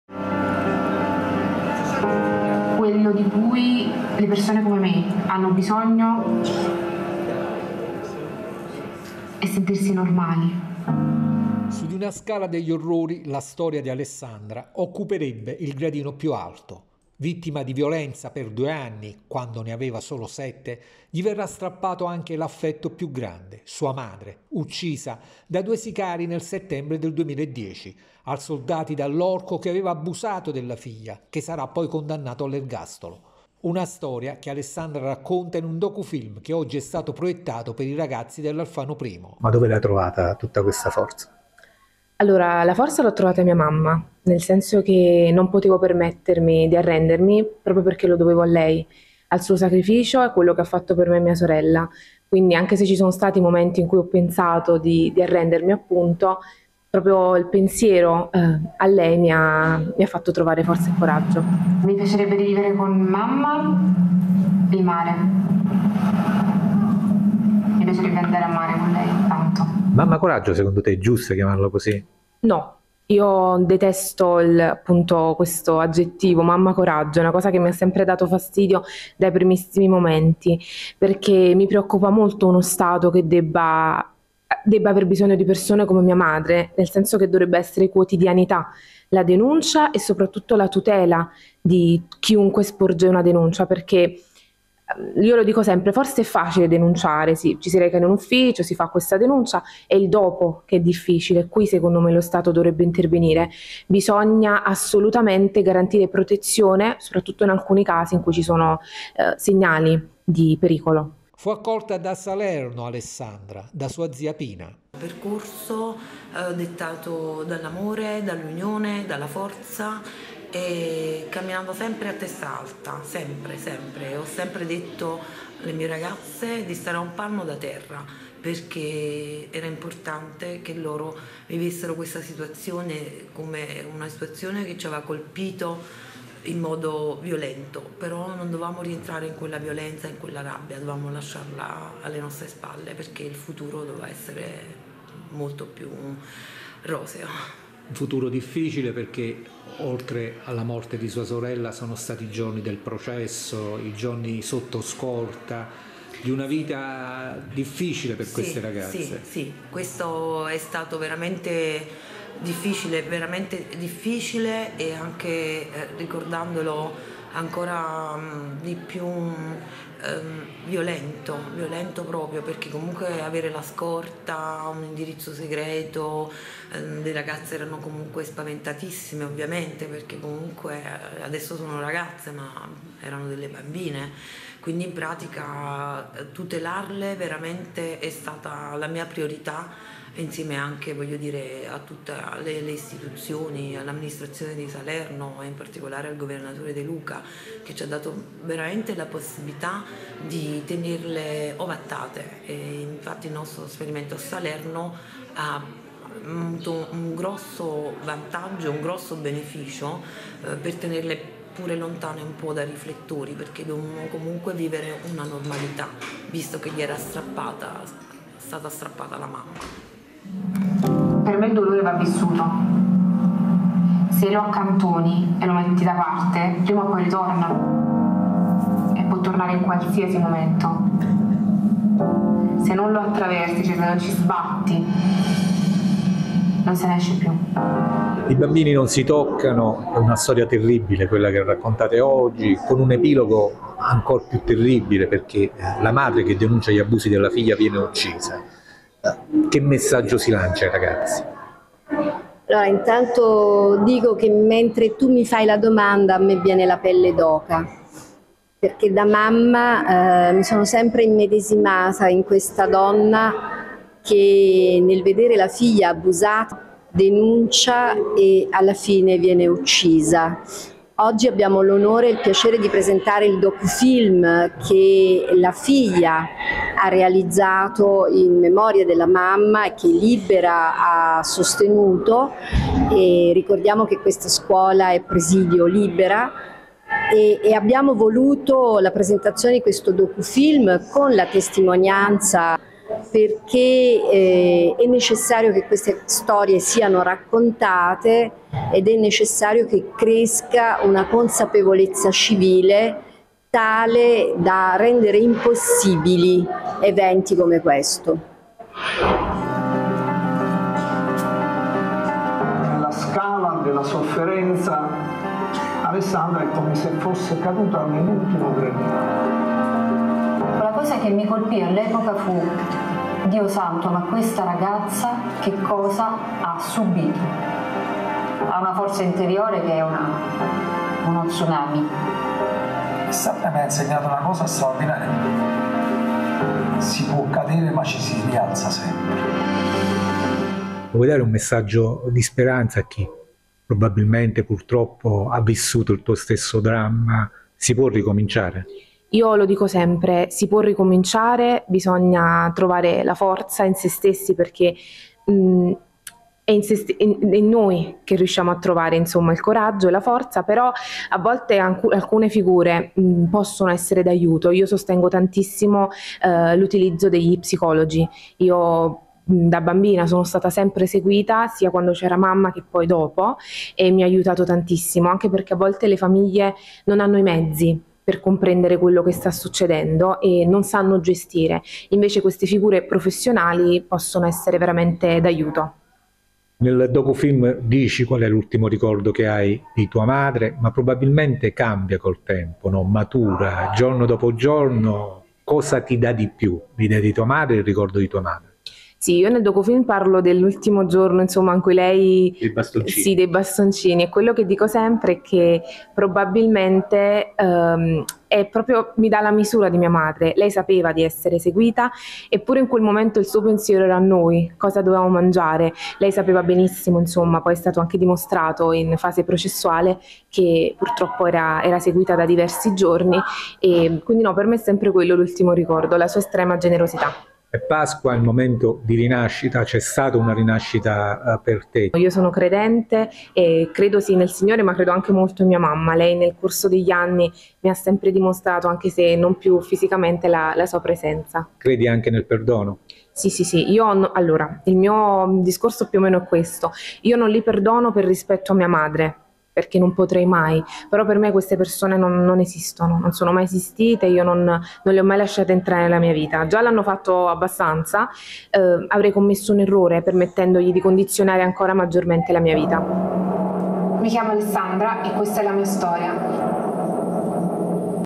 La uccisero per aver denunciato abusi sessuali subiti dalla filglia che ne racconta la storia
Vittima di abusi sessuali, quando aveva solo 7, Anni denunciati dalla madre che venne uccisa per questo. Alessandra raconta agli studenti dell'Alfano I° la sua storia